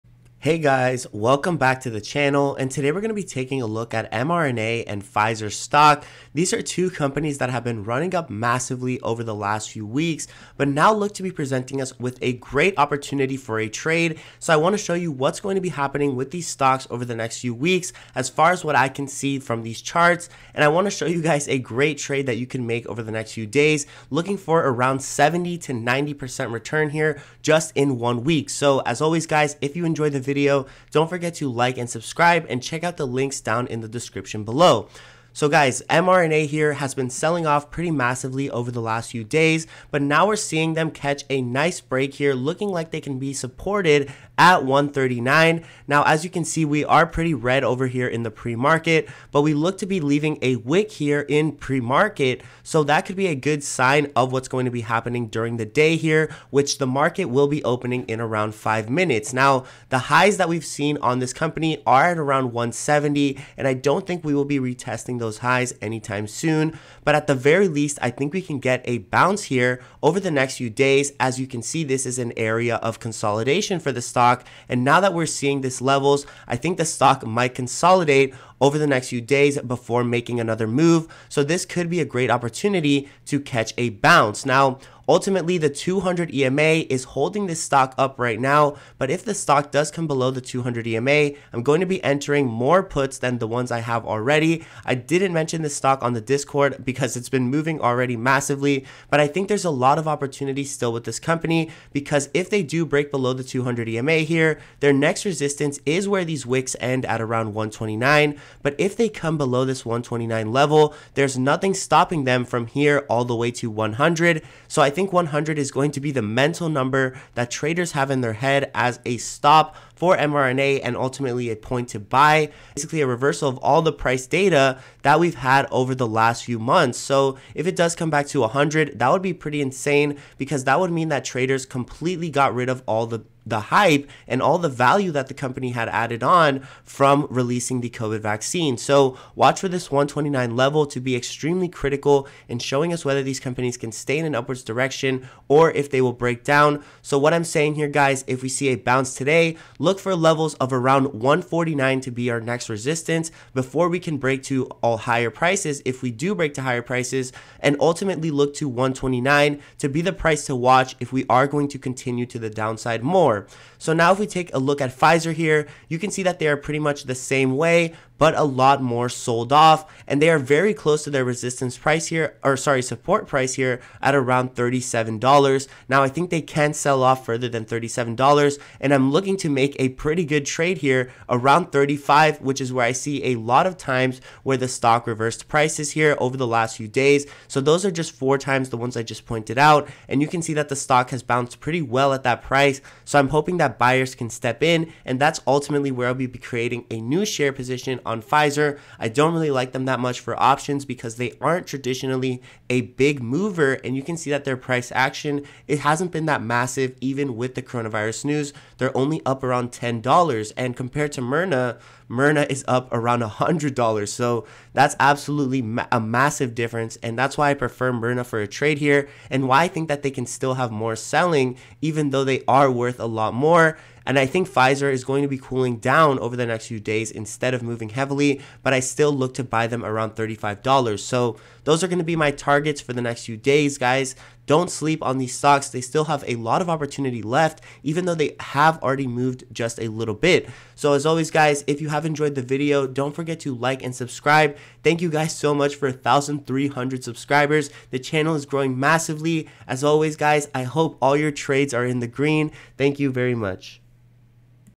MODERNA + PFIZER Stock Analysis (Make Money While At Lows) - Robinhood Investing
Want a free stock? Join Robinhood today!

Got plays you're watching? Put them down in the comments below!